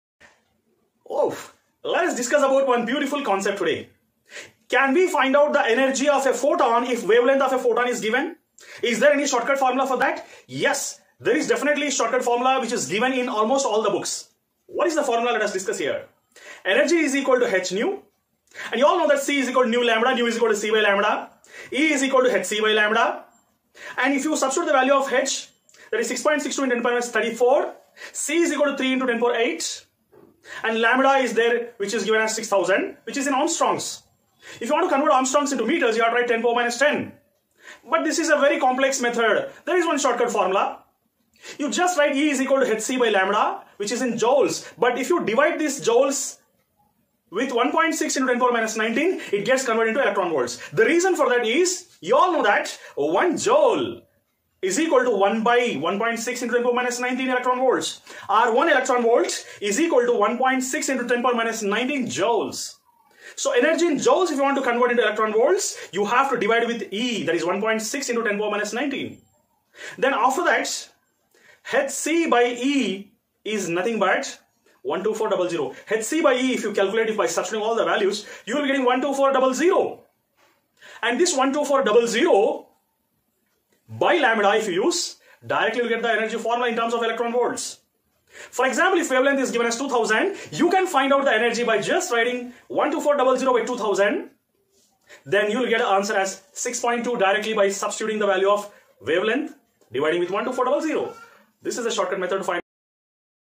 How to find Energy of a photon by using short cut formula | Dual nature of a light
60 days Physics crash course for Neet Jee mains